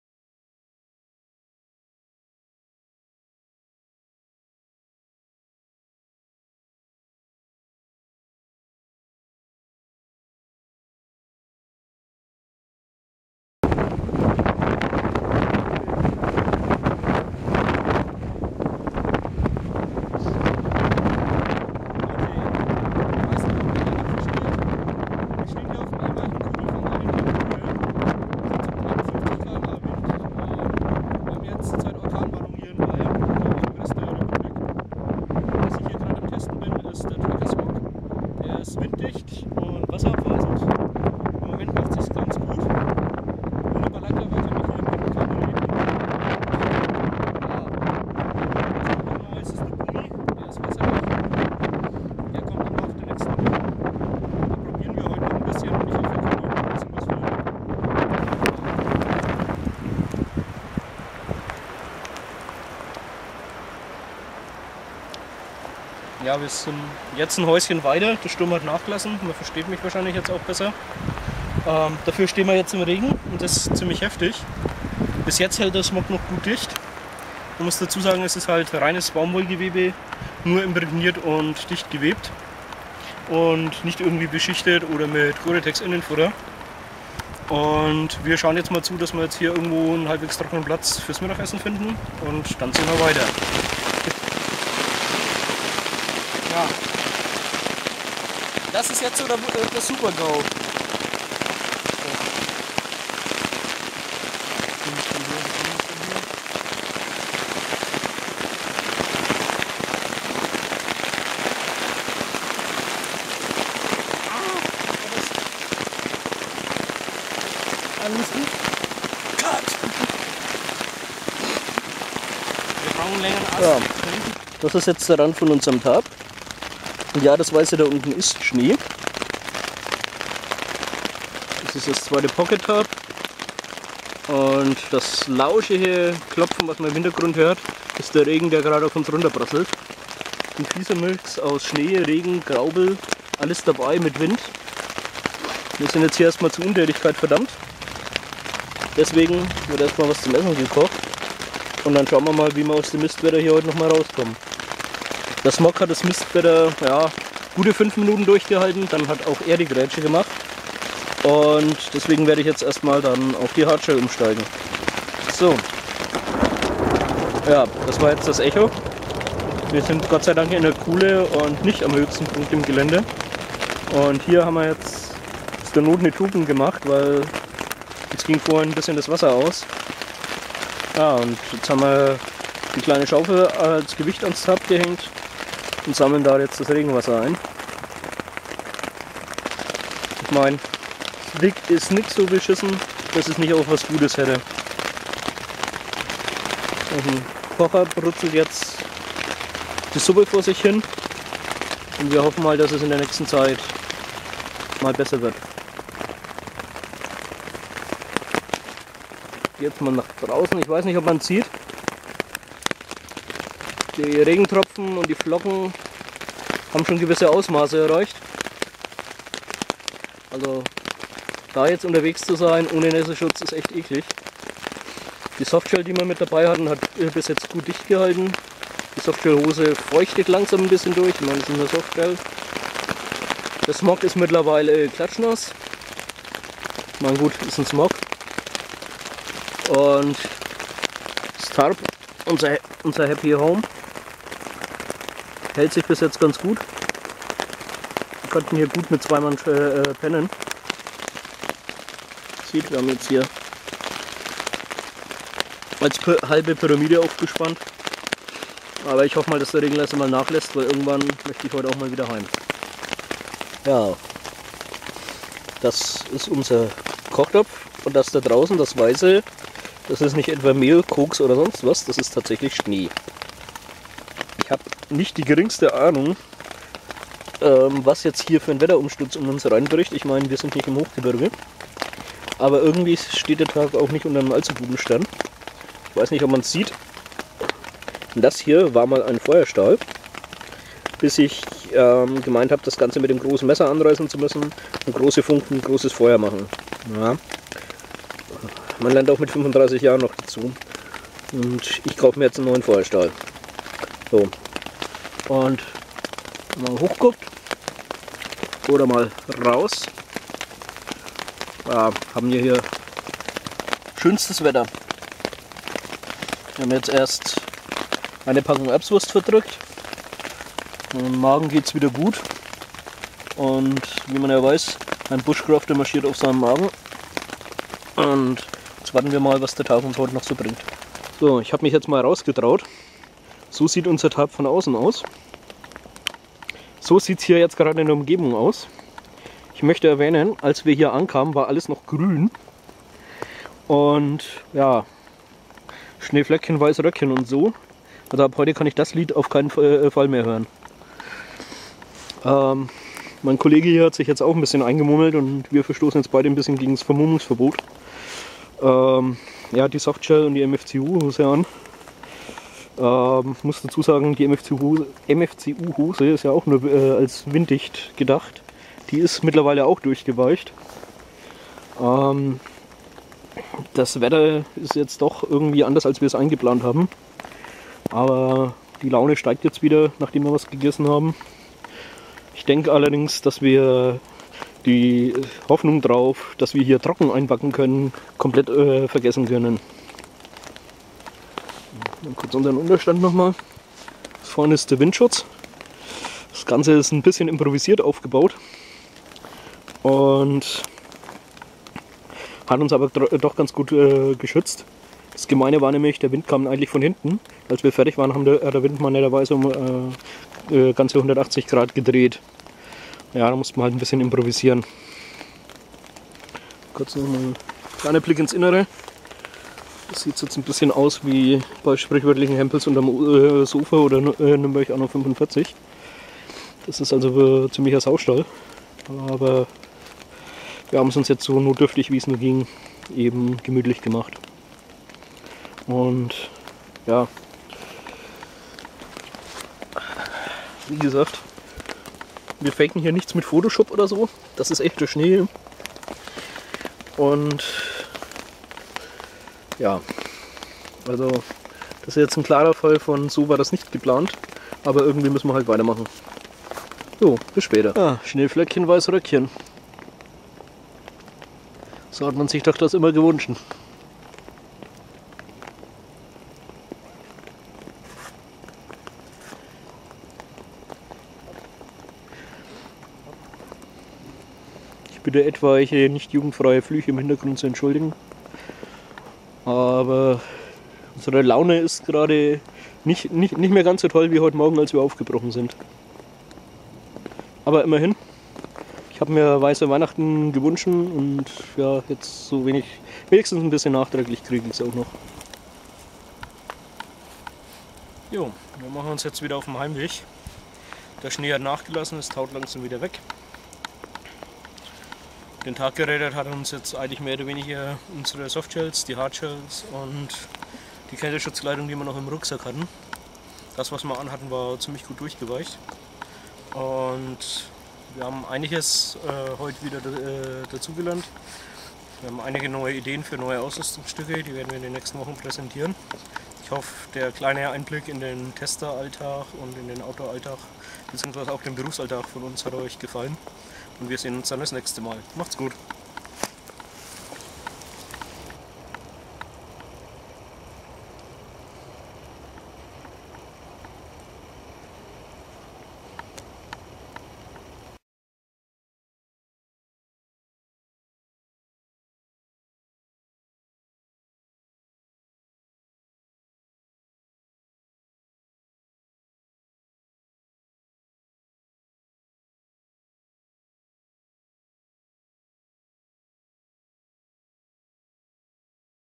Storm Rising
Ein Mitschnitt eines Tages beim Feldtest. Entschuldigt den Schnitt, es war kein vorbereitetes Video, sondern spontane Aufnahmen. Aber ich dachte, es würde euch interessieren.

Packliste:
Bekleidung: BE-X Unterwäsche, BW Socken, Salomon GTX Stiefel, BE-X Balaclava (WIP), BE-X Softshell Boonie, Fleeceshirt, Woolpower Shirt, BE-X Nomex Handschuhe, BE-X Tracker Smock (WIP), Fjällräven Hose, BE-X Hardshell (Muster von 2008)

Im Smock: Schreibzeug, Recta Compass, Survivalkit, Kamera, kl. EH Kit (rat mal wo?), Handy, Stirnlampe, Wasserentkeimtabletten.

In der Hose: Geldbeutel, Taschenmesser Boker+, Feuerstahl, LED, Pfeife, Halstuch, Handschuhe, Merinomütze

Biwak: BE-X Pocket Tarp (BE-X Ranger II Tarp beim Kollegen), BE-X Poncho, BE-X Groundsheet (WIP), Tarpkit (10x Titan Zelthaken, 6 Fixlines, 2 Guylines).

Kochen: Sea-to-Summit Becher, BE-X Polycarbonatflasche (WIP), US Feldbecher, Vargo Titanium Wood Stove Kocher mit Spiritus Brenner, BE-X Titan Spork

Verpflegung: Notration, Erbswurst, Salami, Käse, Tee

Tools: Puma Messer, Boker+ Taschenmesser, Leatherman Tool, BE-X Kettensäge, kleines Beil

Rucksack: Alter TT Daypack

A little clip of our team, during field trials. Excuse the rough cuts, it wasnt a staged shot, but spontanoueus recordings. I though you might be interested.